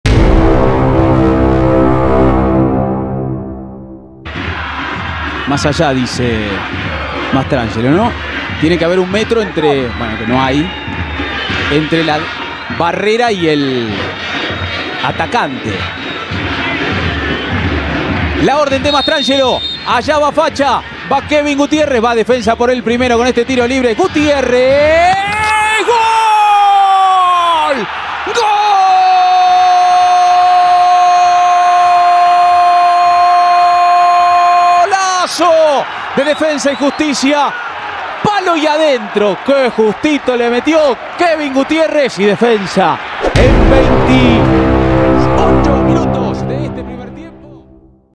Defensa y Justicia [1]-0 Newell's Old Boys - Kevin Gutiérrez 28' (Great Goal)
Kevin Gutiérrez 28' (Great Goal)